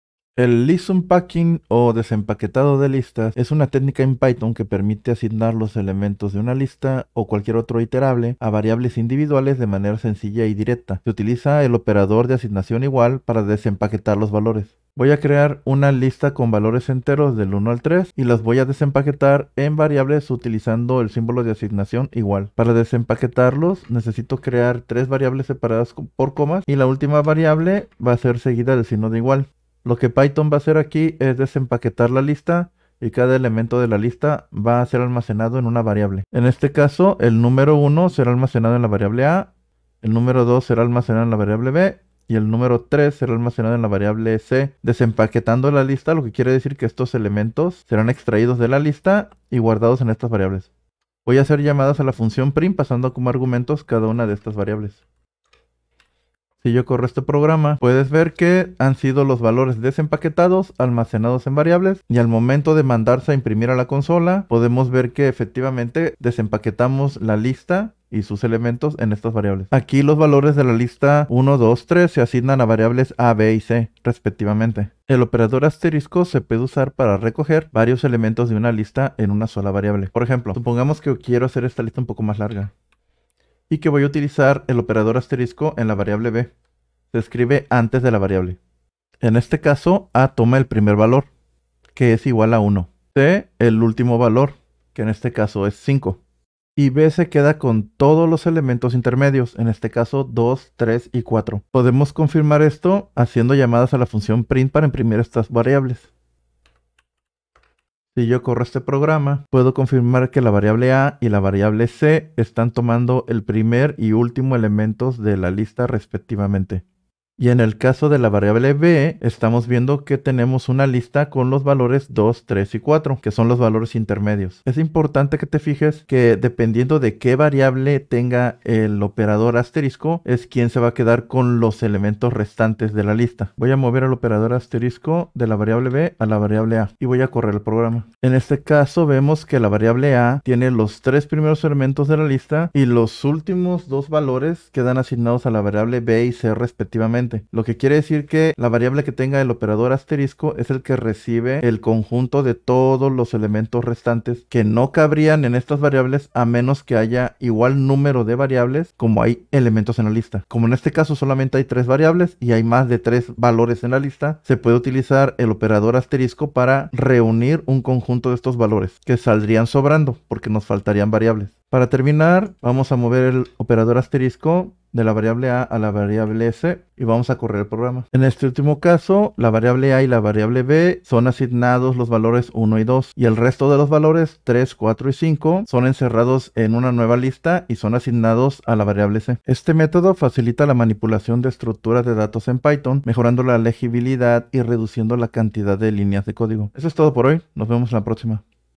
Desempaquetado de listas (list unpacking) en Python
El "list unpacking" en Python es una técnica que permite asignar los elementos de una lista (o cualquier otro iterable) a variables individuales de manera sencilla y directa. Se utiliza el operador de asignación (=) para "desempaquetar" los valores.